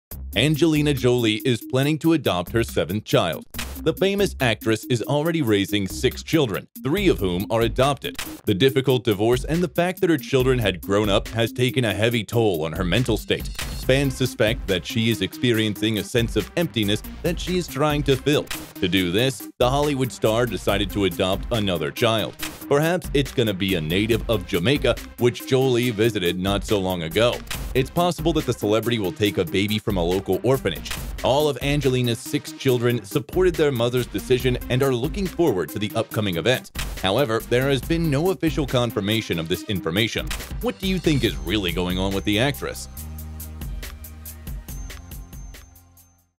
Angelina Jolie is planning to adopt her 7th child! 👶
The famous actress is already raising six children, three of whom are adopted. The difficult divorce and the fact that her children had grown up has taken a heavy toll on her mental state.